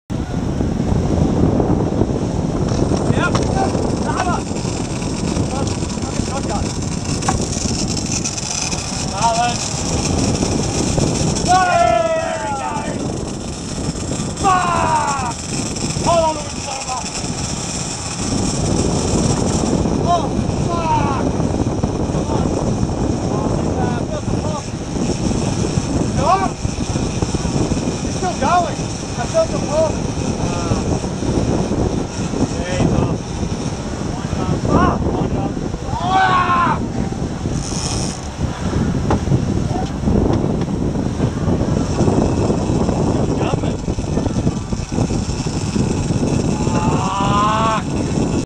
Big Blue Marlin Eats the Shotgun
Never have I lost the top shot that quick!